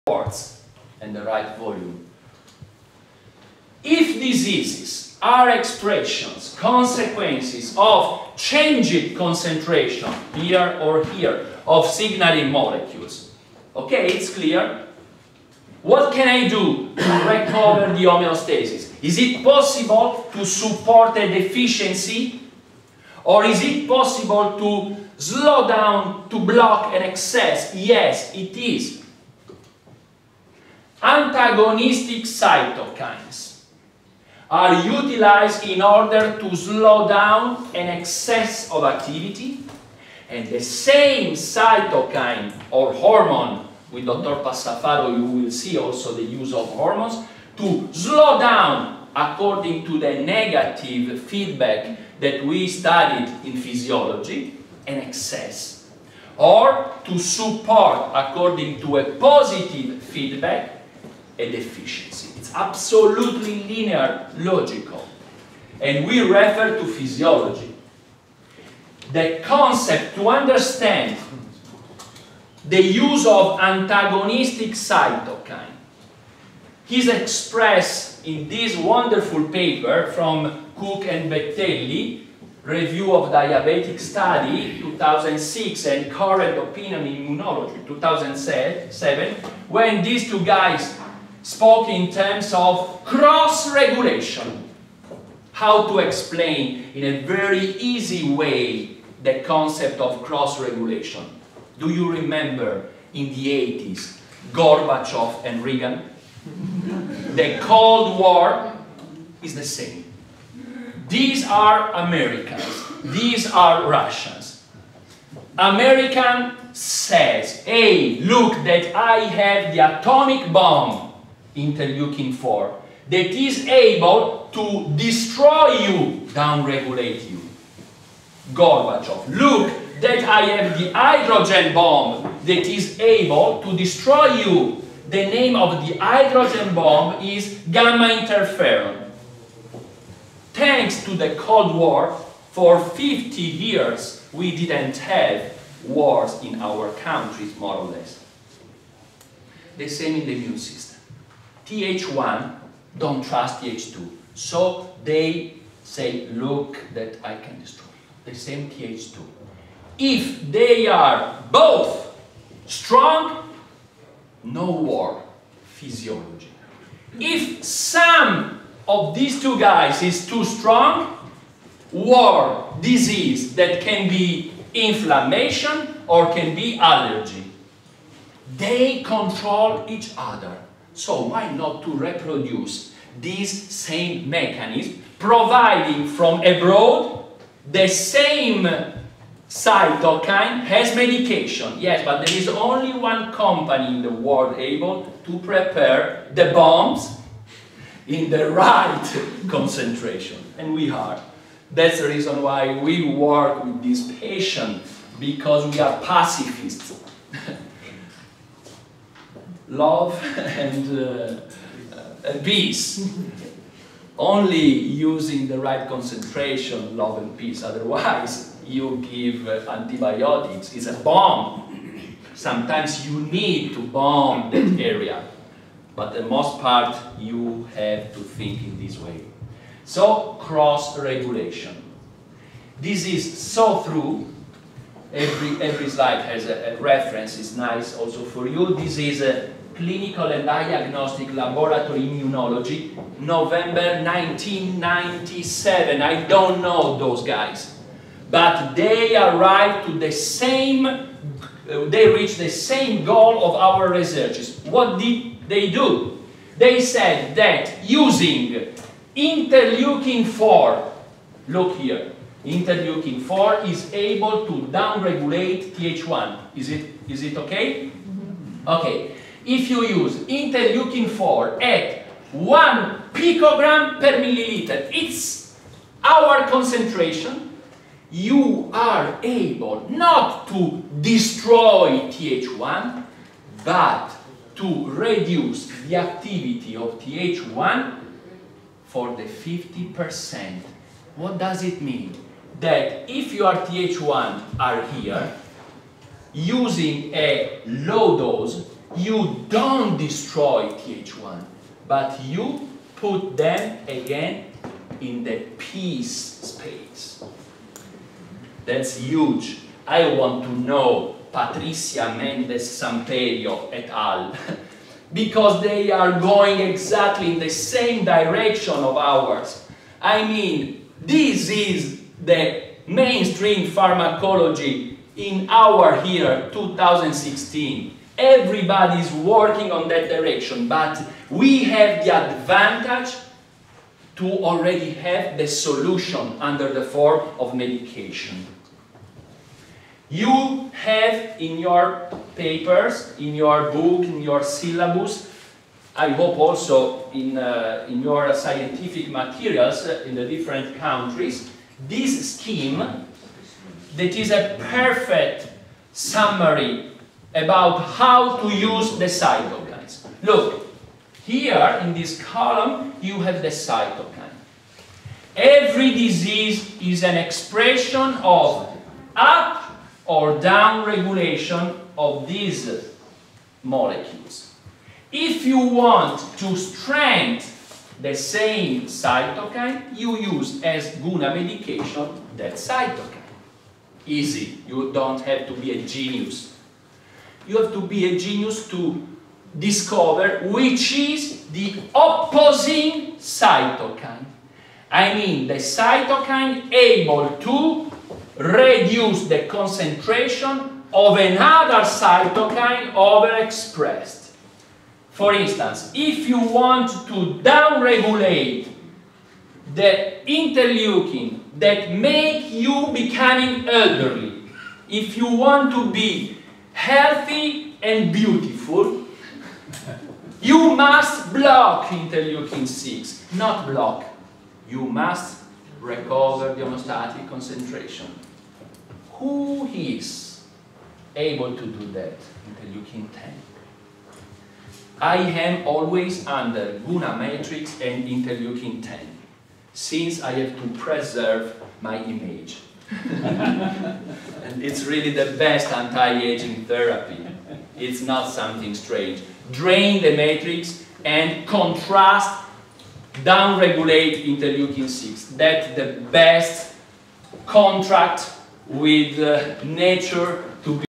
LDM and Concepts of Balance, Up- and Down Regulation. Milan, September 2016
Guna International PRM Seminar 29-30th of September 2016. Ramada Plaza Hotel, Milan, Italy.